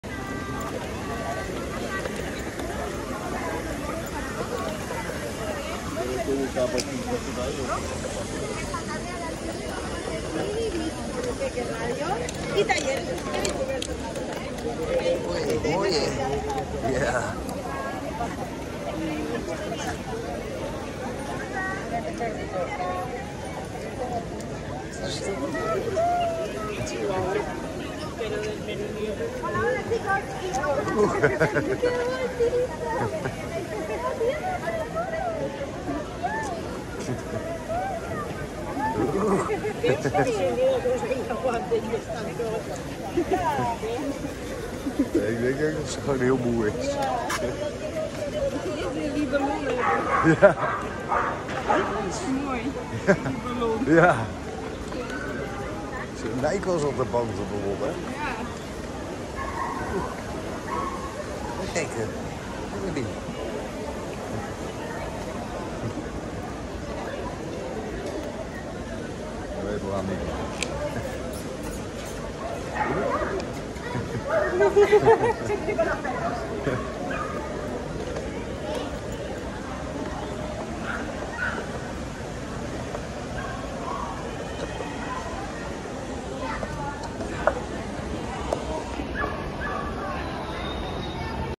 Tiny chihuahua pup @ Petty fest 2022. Making friend and (trying) to steal the balloon 😂 Fiesta time
It was her first Petty fest ever! She met lots of other dogs! She was a little more down then usual since she is losing her puppy teeth 🥹toothfairy